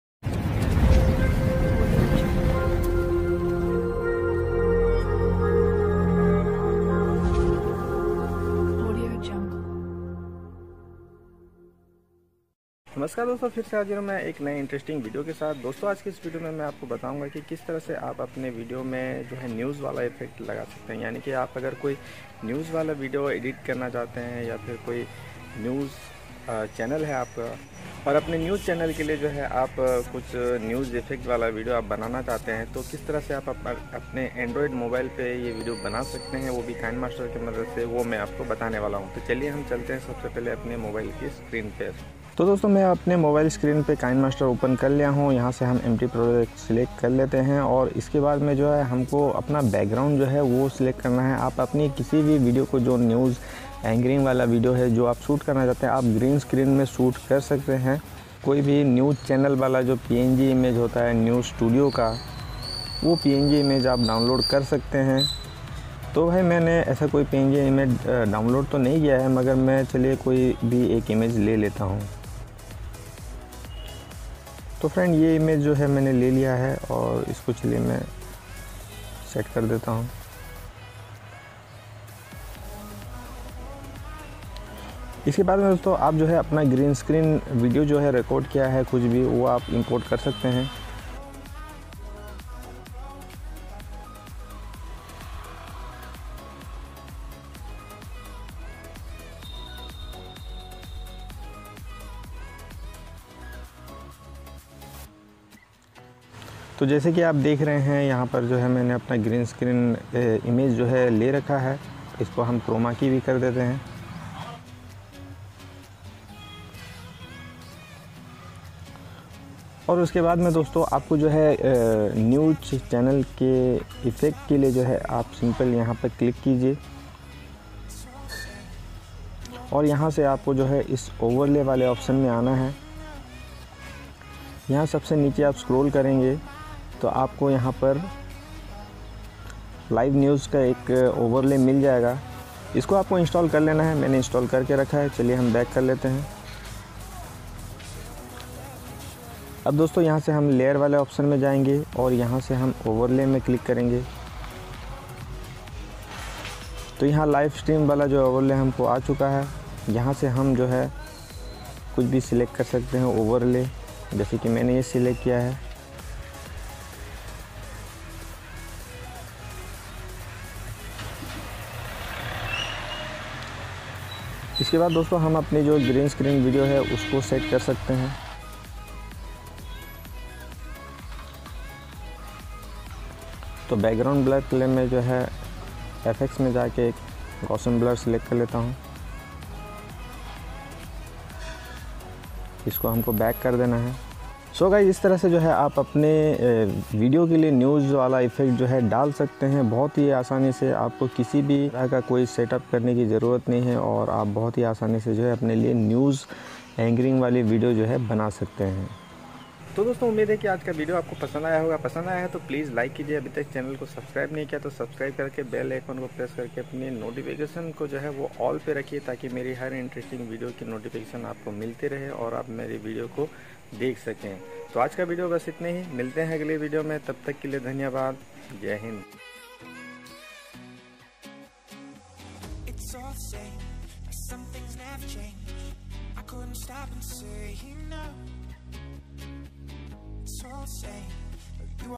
How To Make Videos Like News Channel In Kinemaster | Kinemaster News Channel Video Tutorial
hi friends in this video tutorial i show you how to make videos like news channel in Kinemaster, Kinemaster news channel Video tutorial.

friends if you want to make your home in to home theater buy the best projecter like me visitek V6 Projecter. purchase link -

2- E-GATE K9 PROJECTER

kinemaster latest mod apk 2020